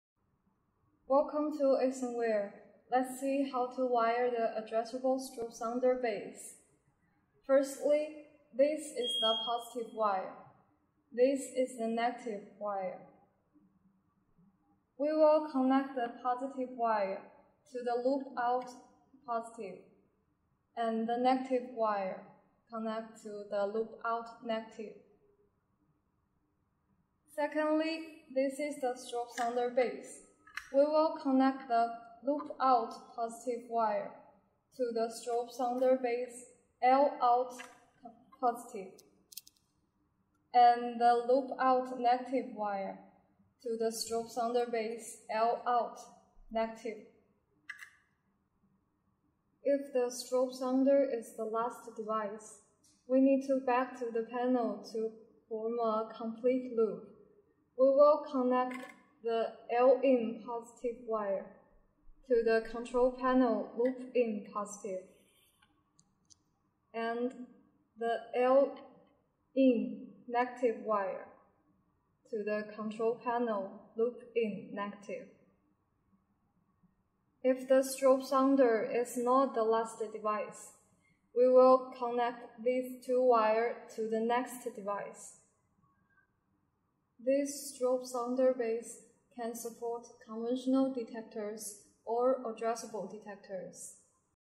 How To Wire Addressable Detector Strobe Sounder Base
In This Video Asenware Fire Alarm System Technician Shows How To Wire Strobe Sounder Base To Addressable Fire Alarm Control Panel